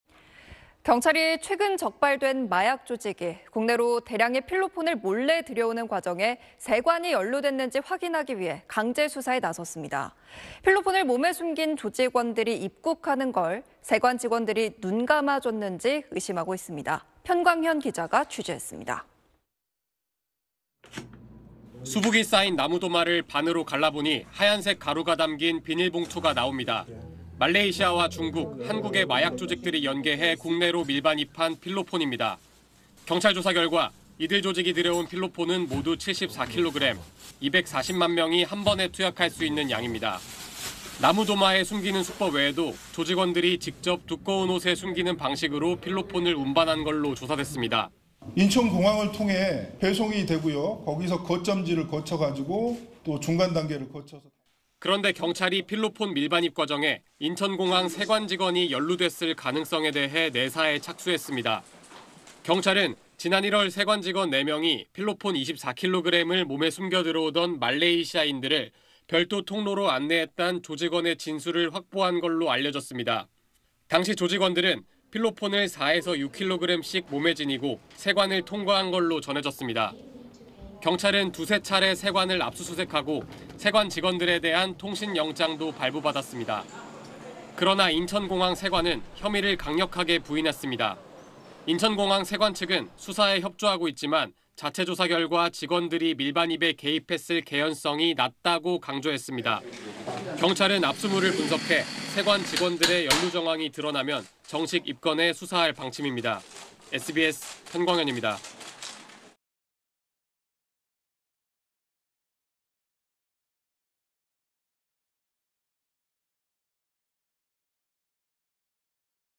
나무 도마 갈라봤더니…"별도 통로로 안내했다" / SBS 8뉴스
〈앵커〉

경찰이 최근 적발된 마약조직이 국내로 대량의 필로폰을 몰래 들여오는 과정에 세관이 연루됐는지 확인하기 위해 강제수사에 나섰습니다. 필로폰을 몸에 숨긴 조직원들이 입국하는 걸 세관 직원들이 눈 감아 줬는지 의심하고 있습니다.

편광현 기자가 취재했습니다.

〈기자〉

수북이 쌓인 나무 도마를 반으로 갈라보니 하얀색 가루가 담긴 비닐 봉투가 나옵니다.

말레이시아와 중국, 한국의 마약 조직들이 연계해 국내로 밀반입한 필로폰입니다.

경찰 조사 결과 이들 조직이 들여온 필로폰은 모두 74kg, 240만 명이 한 번에 투약할 수 있는 양입니다.

나무 도마에 숨기는 수법 외에도 조직원들이 직접 두꺼운 옷에 숨기는 방식으로 필로폰을 운반한 것으로 조사됐습니다.

[백해룡/영등포경찰서 형사2과장 : 인천공항을 통해 배송이 되고요. 거기서 거점지를 거쳐 가지고 또 중간 단계를 거쳐서….]

그런데 경찰이 필로폰 밀반입 과정에 인천공항 세관 직원이 연루됐을 가능성에 대해 내사에 착수했습니다.

경찰은 지난 1월 세관 직원 4명이 필로폰 24kg을 몸에 숨겨 들여오던 말레이시아인들을 별도 통로로 안내했다는 조직원의 진술을 확보한 것으로 알려졌습니다.

당시 조직원들은 필로폰을 4∼6kg씩을 몸에 지니고 세관을 통과한 것으로 전해졌습니다.

경찰은 두세 차례 세관을 압수수색하고, 세관 직원들에 대한 통신영장도 발부받았습니다.

그러나, 인천공항 세관은 혐의를 강력하게 부인했습니다.

인천공항 세관 측은 "수사에 협조하고 있지만, 자체 조사 결과 직원들이 밀반입에 개입했을 개연성이 낮다"고 강조했습니다.

경찰은 압수물을 분석해 세관 직원들의 연루 정황이 드러나면 정식 입건해 수사할 방침입니다.

(영상취재 : 윤 형, 영상편집 : 오영택)